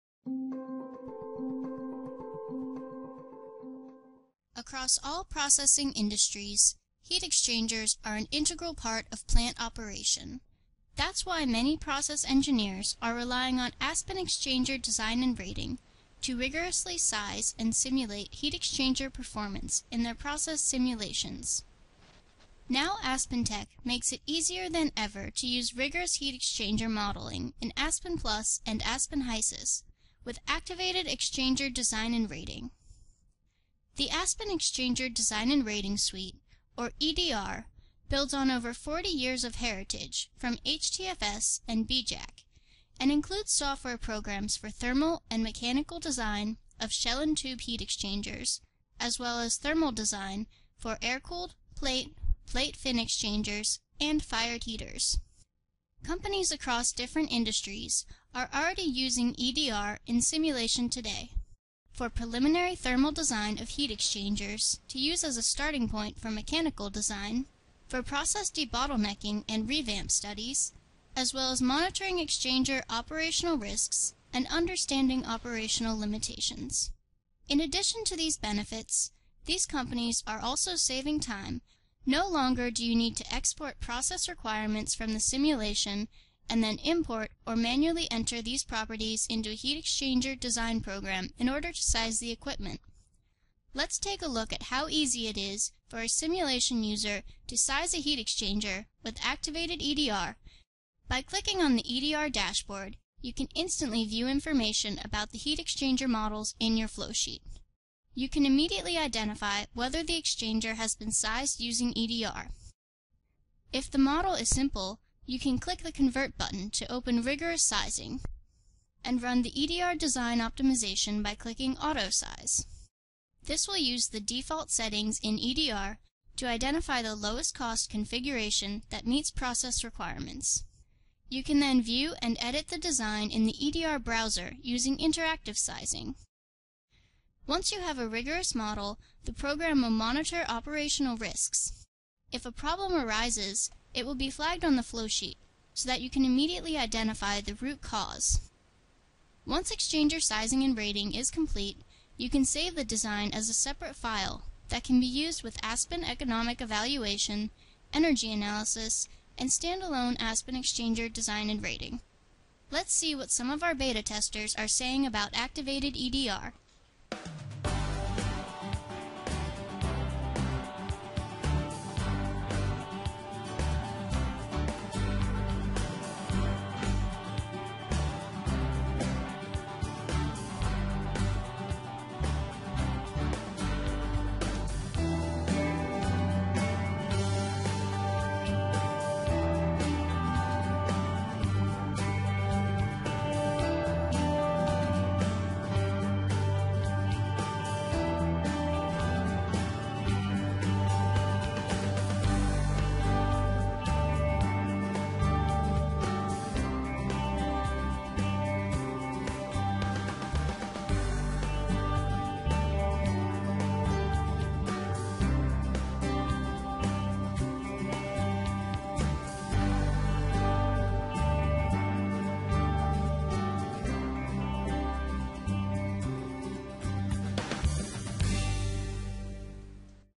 Activated Exchanger Design & Rating
This video demonstrates the new Activated Exchanger Design & Rating (EDR) functionality available in Aspen Plus and Aspen HYSYS. Activated EDR makes it easier to utilize rigorous exchanger models in simulation, as well as identify and troubleshoot operational risks and warnings in exchanger performance.